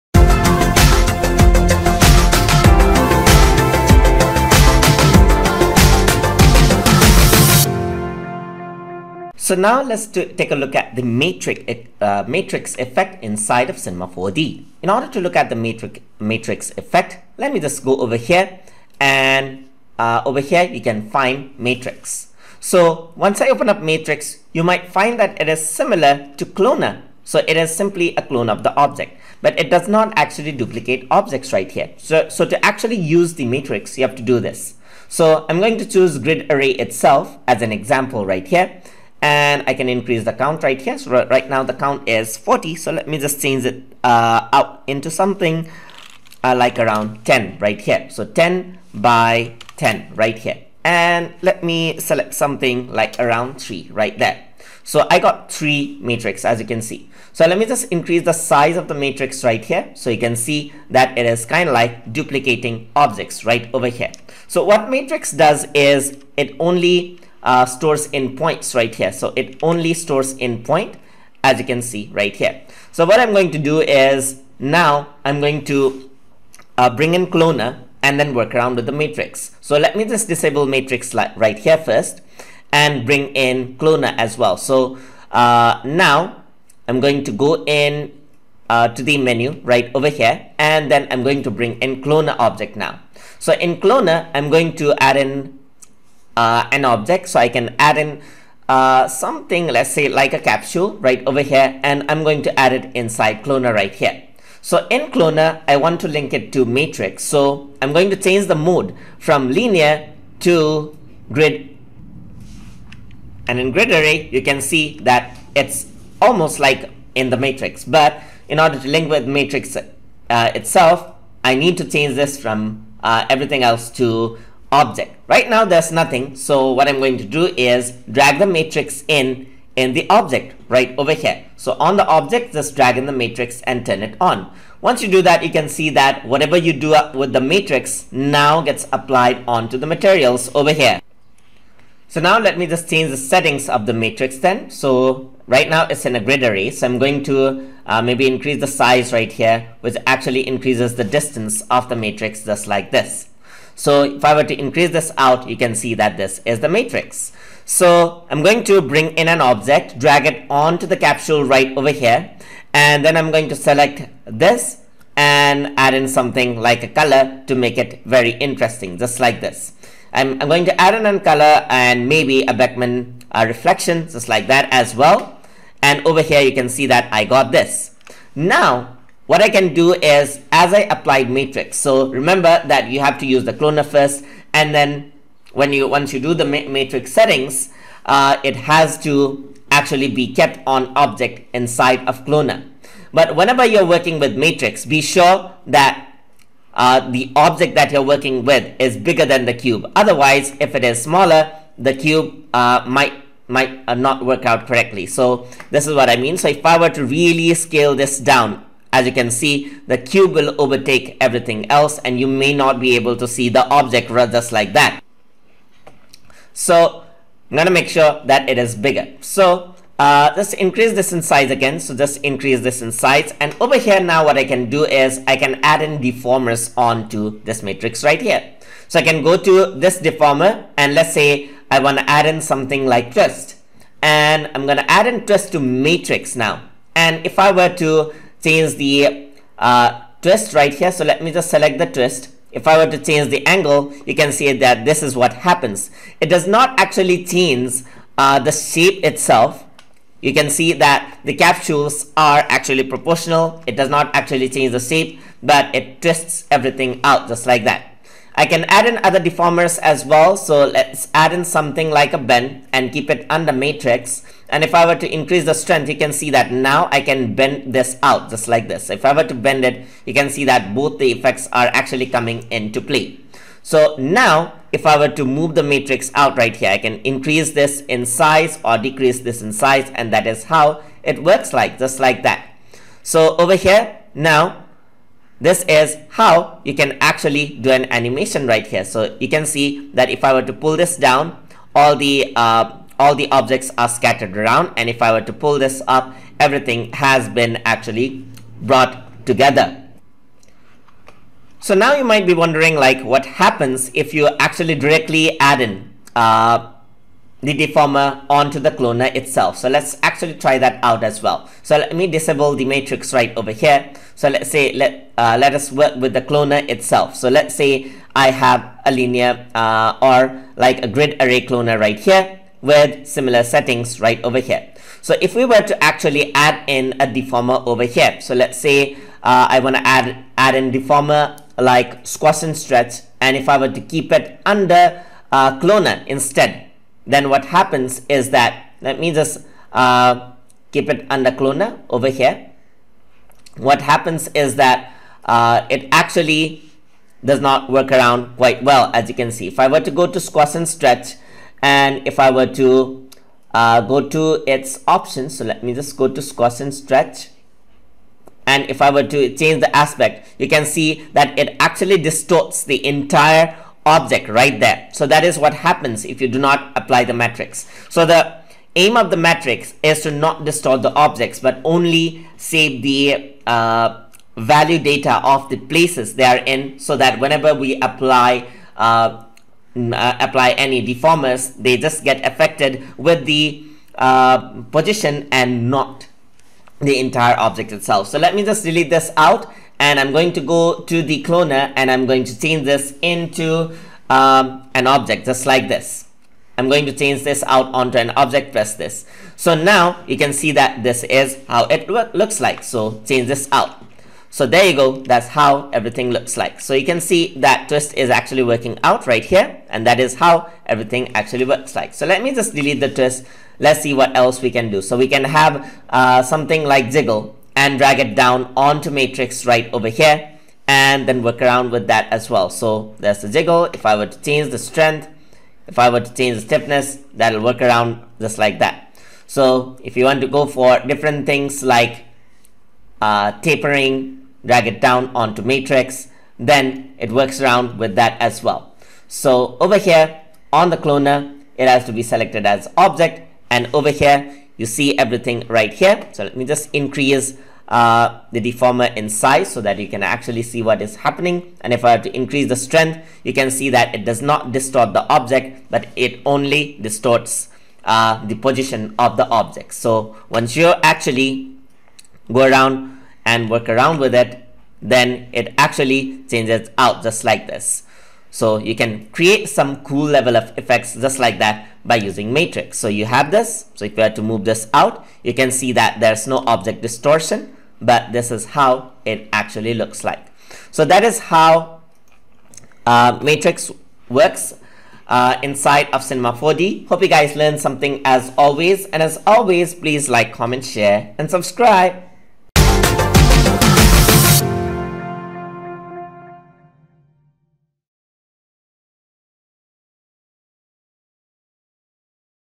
Cinema 4D Tutorial - Lesson 117 - Matrix
In this tutorial, we will be discussing about Matrix in Cinema 4D.